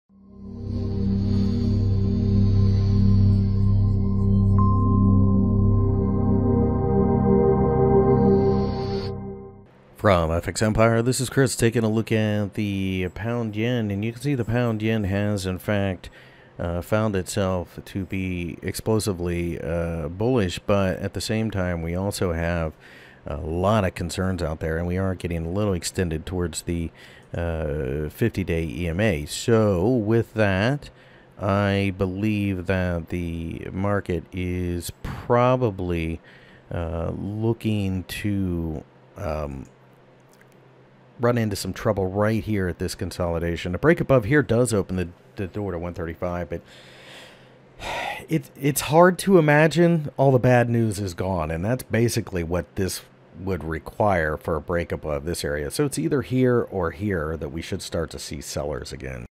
GBP/JPY Technical Analysis for September 06, 2019 by FXEmpire
After the nonsense going on in London, there seems to be a general “risk on” attitude when it comes to the British pound. For the full article go - GBP/JPY Forecast September 06, 2019, Technical Analysis.